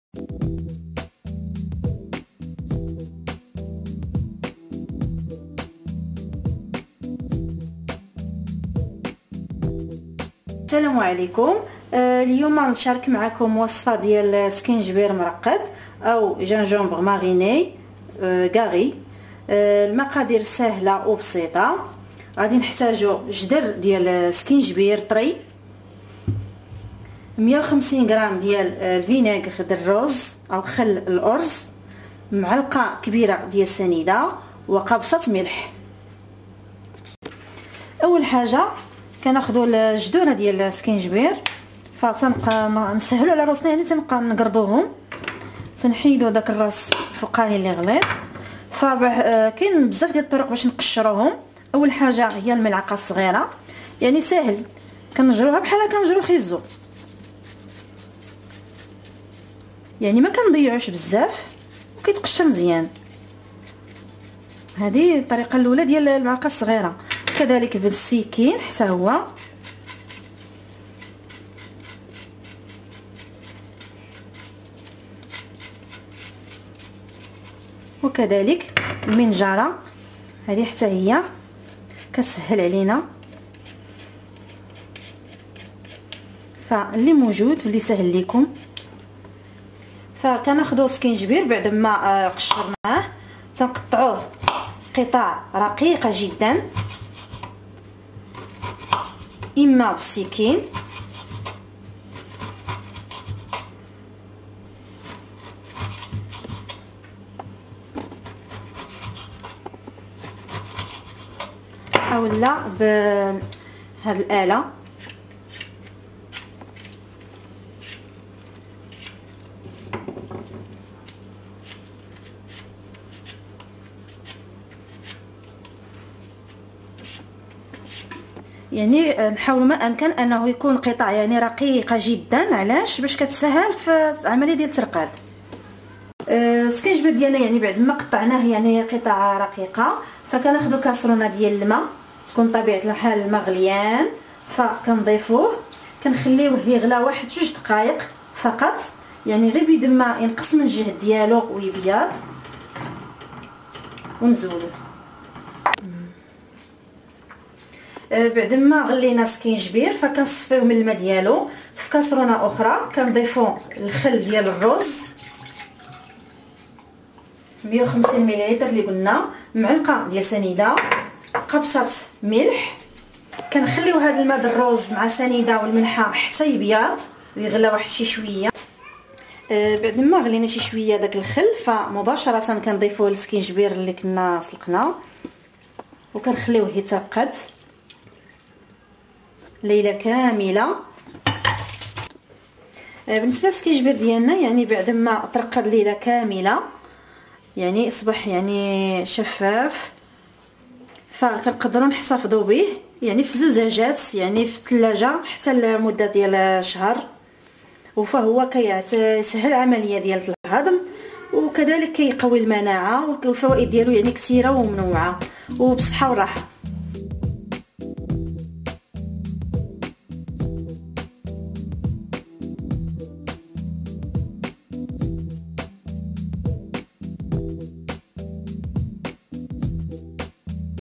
gingembre mariné gari الزنجبيل المتبل
Ingrédients
1 Gingembre
150 ml de vinaigre de Riz
1 CAS de sucre Granulé
1 pincée de sel